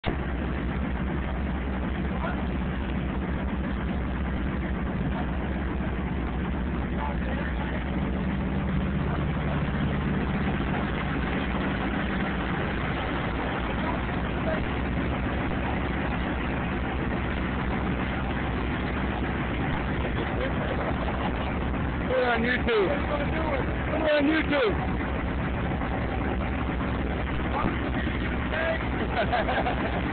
England Driver's Bad Day In Chicago..
Rookie England Lease Operator runs out of fuel and is towed from Chicago loading dock..